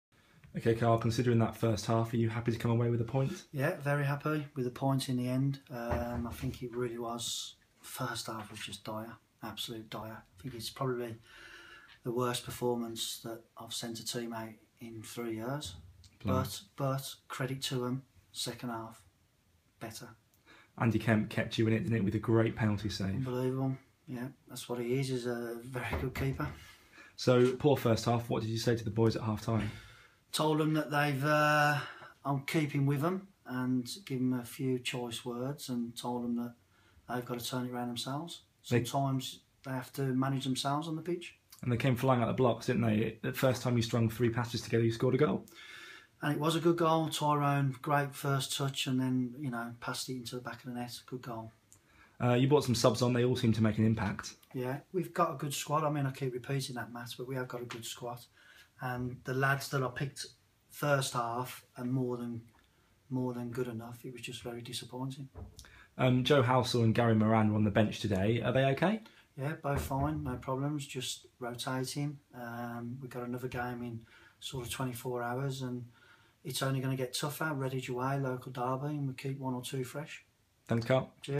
Carl Adams speaks after Stratford Town 1 Chesham United 1
Stratford Town's manager speaks to Herald Sports Editor, Matt Wilson, after a draw in the Southern League Prem. 29/08/15